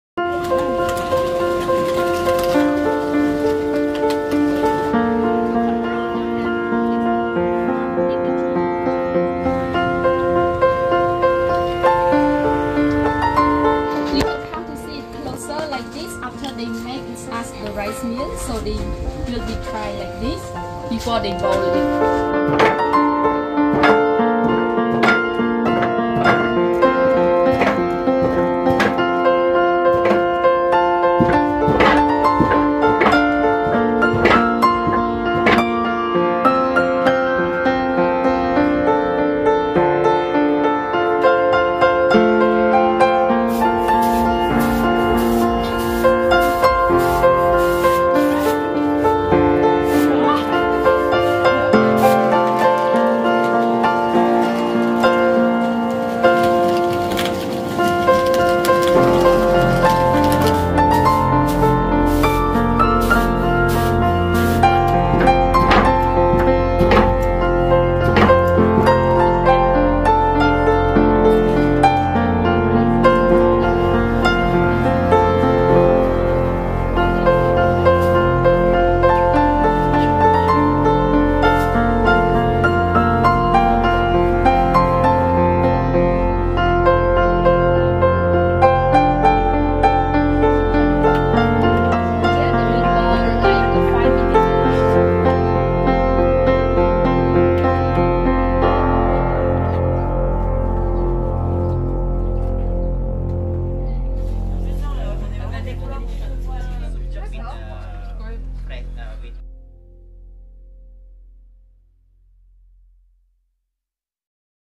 Raid Amazones Women Immerse in Local Lifestyle
SIEM REAP - After a 15 km trail running, participants of Raid Amazones learned about the livelihoods of the local people in Chub village, Khnar Po commune, Soutr Nikum district.

The participants offered study materials to students at Romdeng Primary School and learned how to grind the rice grains and rid them of husk and bran. They also got to taste the freshly-made traditional Khmer sticky noodle.

Note: This video was uploaded to Cambodianess Facebook page on March 15, 2024.